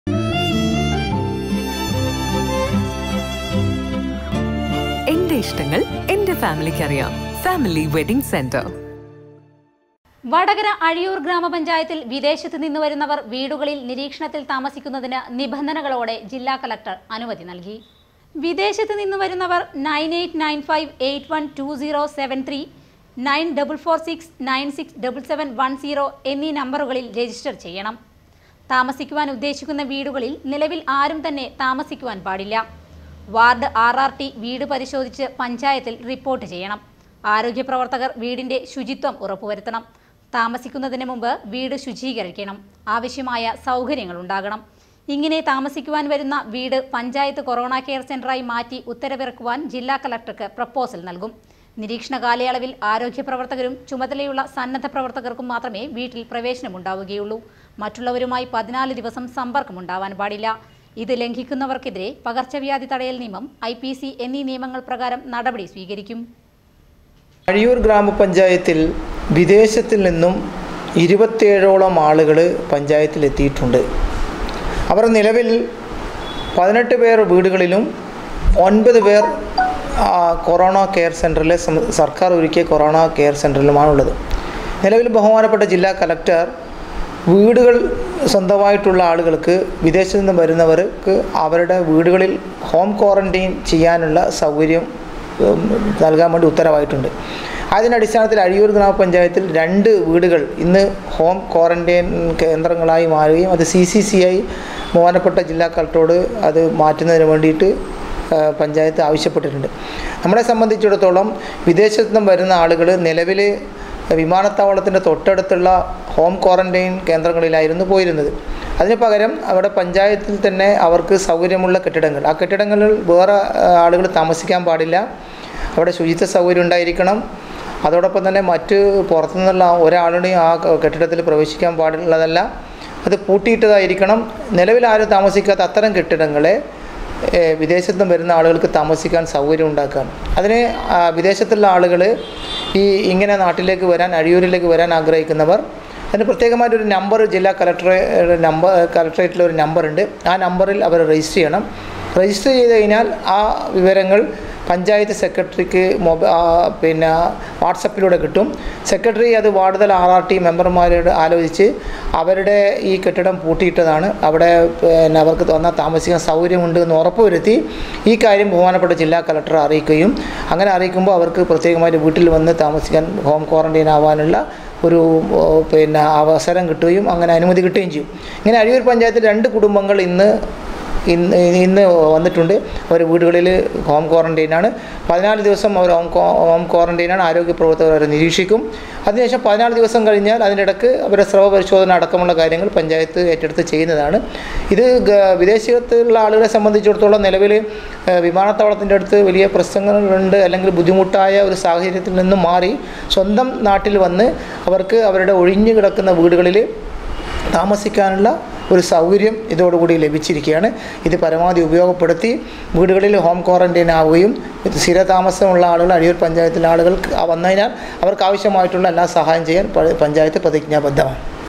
വീടുകളിൽ നിരീക്ഷണത്തിൽ താമസിക്കുന്നതിന് നിബന്ധനകളോടെ ജില്ലാ കളക്ടർ അനുമതി നൽകി
വടകര അഴിയൂർ ഗ്രാമ പഞ്ചായത്തിൽ വിദേശത്തു നിന്ന് വരുന്നവർ വീടുകളിൽ നിരീക്ഷണത്തിൽ താമസിക്കുന്നതിന് നിബന്ധനകളോടെ ജില്ലാ കളക്ടർ അനുമതി നൽകി